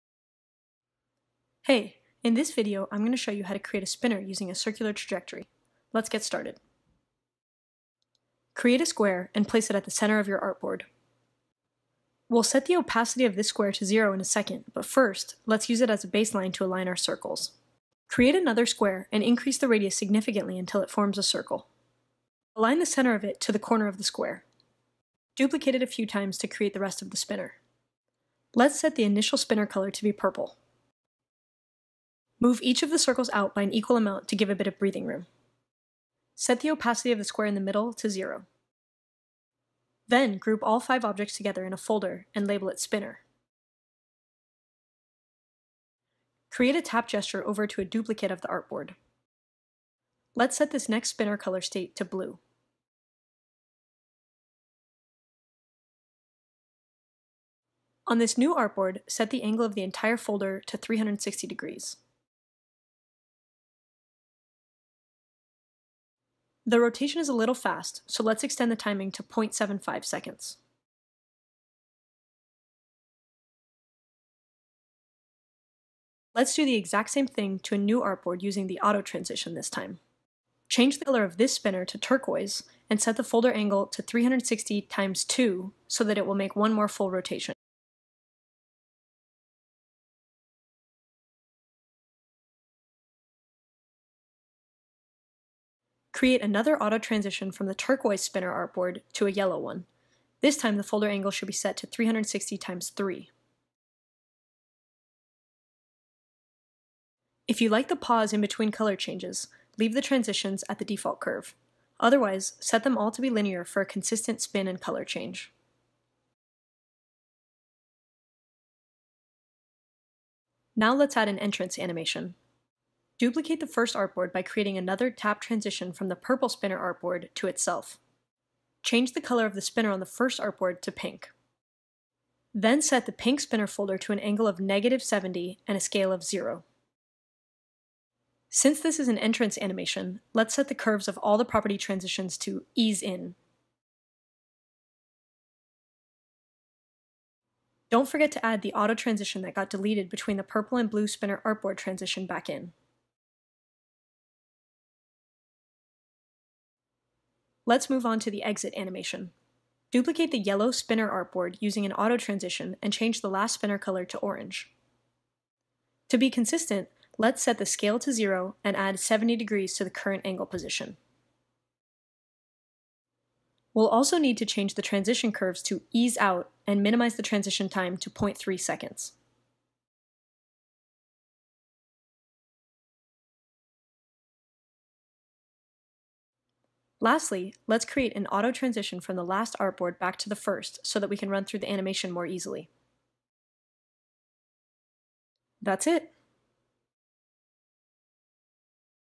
Principle Tutorial Circular Motion Spinner
This tutorial will teach you how to apply circular motions to objects in Principle.

In this case, we’ll be creating a simple spinner that changes color as it rotates. Creating circular trajectories isn’t even mentioned in Principle’s official documentation, but it’s a pretty easy hack. We’ll start by placing an invisible shape at the center of the four circles that make up our spinner. We’ll then group all five objects together (the four circles plus the invisible shape) into a folder, which will allow us to create a circular motion using angle rotations between artboards.

Rotating the entire folder by 360 degrees will make one full clockwise circle. Rotating it by 720 degrees (360 x 2) will make two full circles. And rotating it by negative 360 degrees will make one complete loop in the other direction (counter-clockwise).

The main takeaway of this tutorial is this: As long as your objects are grouped together in a folder, you can use rotation angles to create circular trajectories.

No assets are needed for this tutorial since you can create them all directly in Principle.